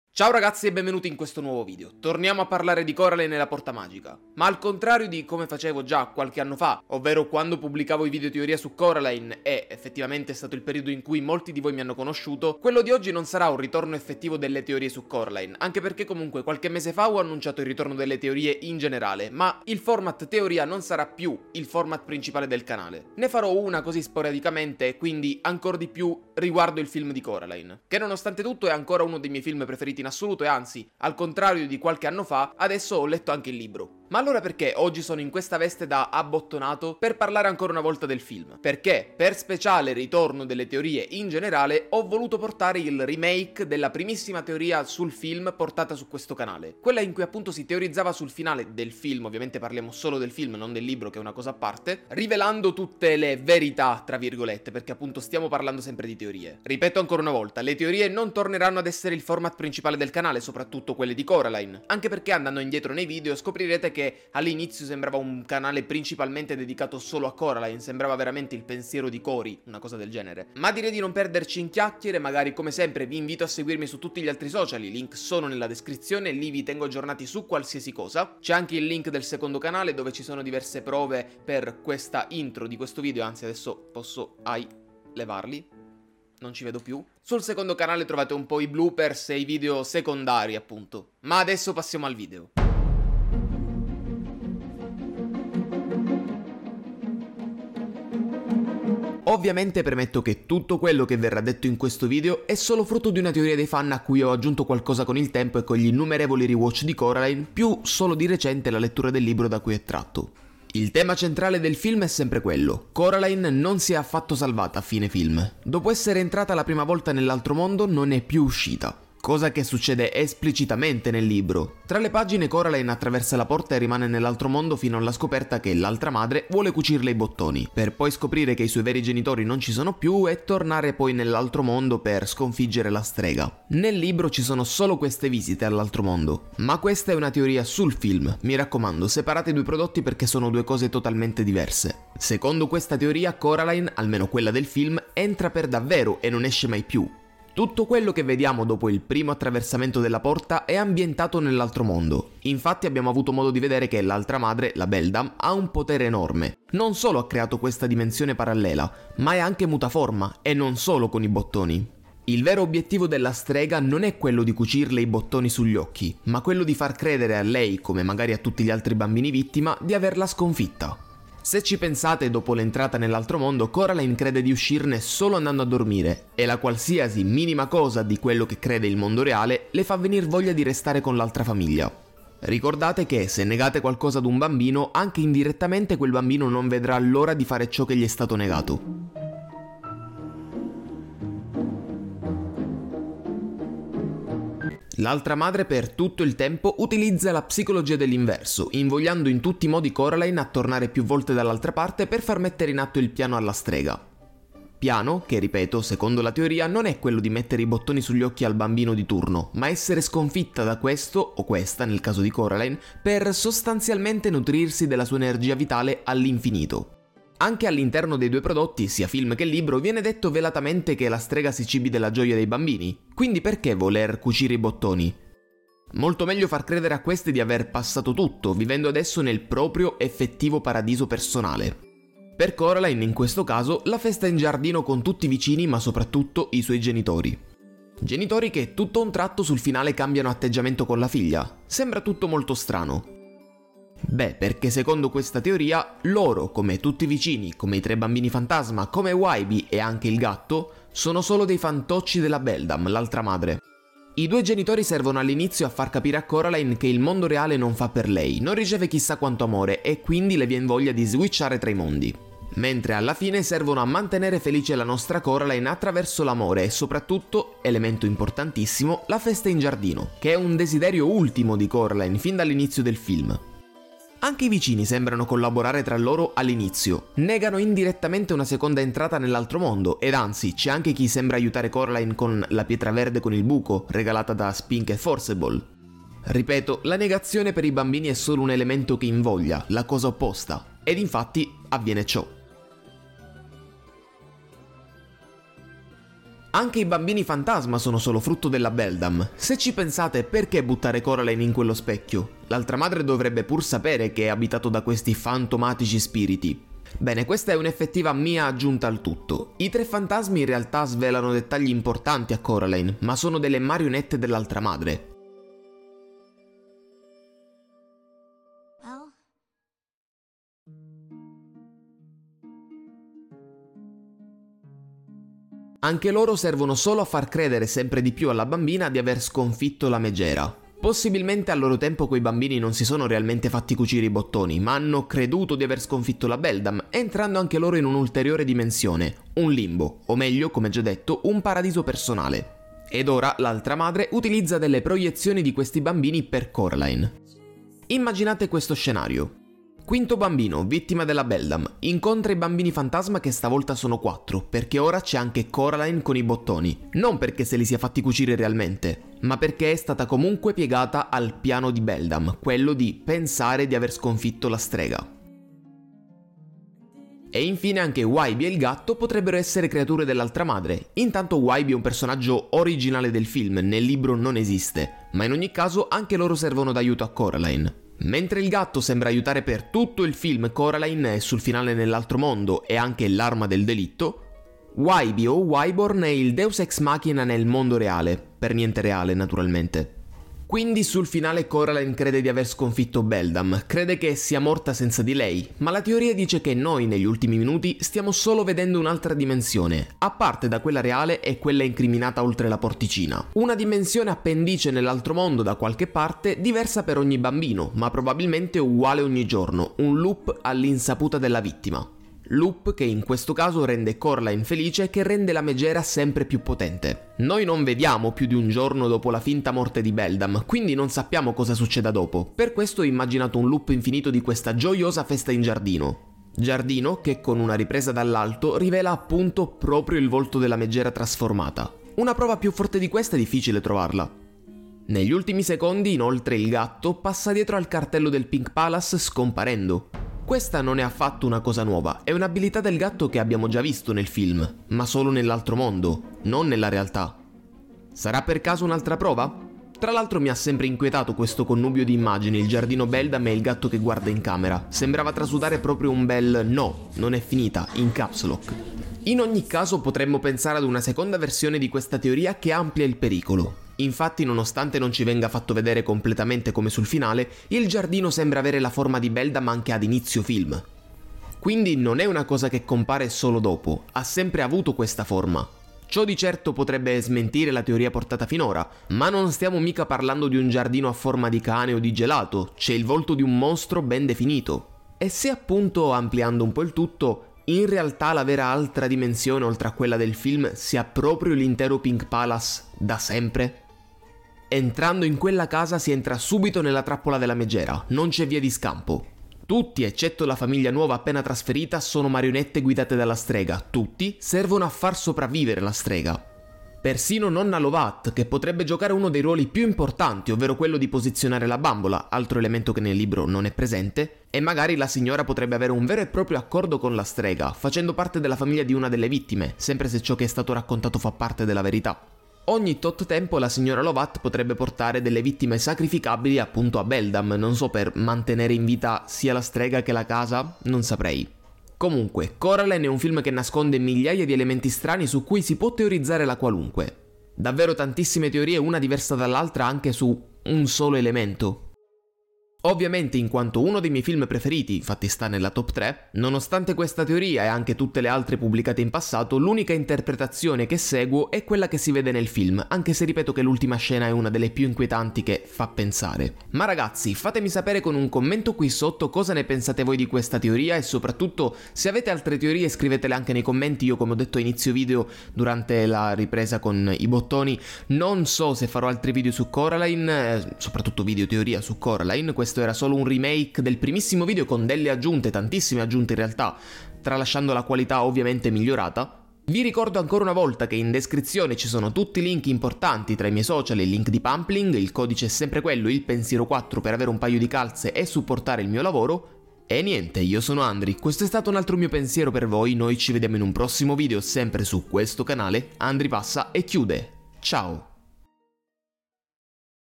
IL VERO FINALE DI CORALINE - TEORIA
Ciao ragazzi, benvenuti tra i miei pensieri.
Dopo più di quattro anni ecco il remake del primissimo video teoria su Coraline con tante nuove aggiunte arrivate con il tempo e con altri innumerevoli rewatch del magnifico film in stop motion.
La qualità è leggermente differente da quella di marzo 2019...

𝗖𝗼𝗺𝗽𝗿𝗮 𝗶𝗹 𝗹𝗶𝗯𝗿𝗼 𝗮𝗱 𝘂𝗻 𝗽𝗿𝗲𝘇𝘇𝗼 𝘀𝗰𝗼𝗻𝘁𝗮𝘁𝗼: